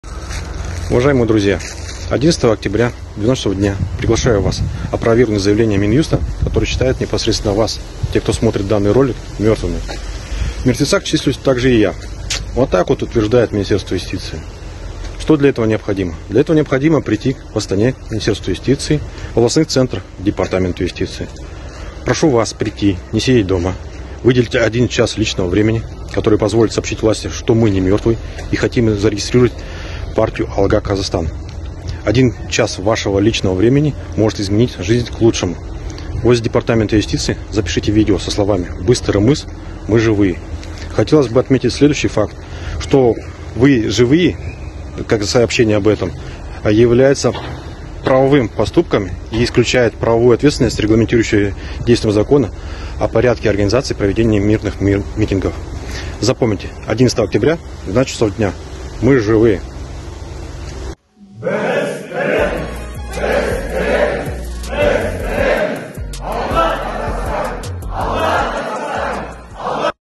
СЕРГЕЙ ИЗМАЙЛОВ: ПРИГЛАШАЮ ОПРОВЕРГНУТЬ НЕЗАКОННОЕ ЗАЯВЛЕНИЕ МИНИСТЕРСТВА ЮСТИЦИИ!
Живые мертвецы.

Сейчас эта воровская и авторитарная власть всех,  кто не согласен с этим антинародным режимом,  считает за террористов и мертвецов.

Почему, скажете вы?

Потому что миллионы жителей нашей страны не хотят мириться с этой несправедливостью и борются с этой гидрой.

Выйдешь на митинг с требованием справедливости,  расстреляют и объявят террористом.

Хочешь зарегистрировать партию, объявят мертвецом.

Вот она Токаевская трусливая и лживая власть.

Кто не хочет быть живым мертвецом и живым рабом этого режима, пусть выходит к министерству юстиции в Астане и к департаментам юстиции областей и выразит нам свою поддержку в регистрации нашей партии!

Мы должны доказать, что нас миллионы и все мы хотим изменений и реформ.

Мы изменим этот режим. Добьёмся перемен и процветания!

АлғаҚазақстан!